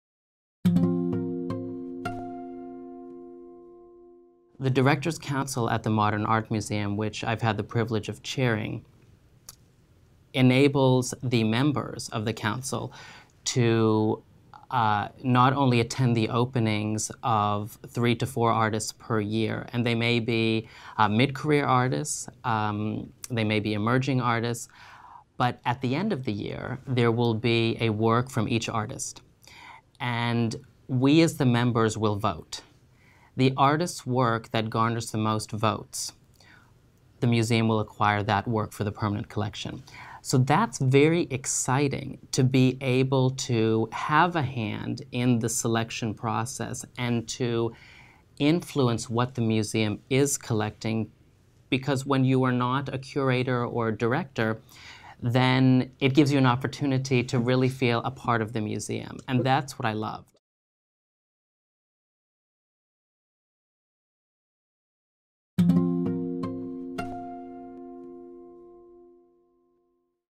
Preview: Modern Art Museum of Fort Worth - Asad Dean
Dr. Asad Dean, Board Member of the Modern Art Museum of Fort Worth, discusses the inclusive process of selecting artwork for the museum’s collection.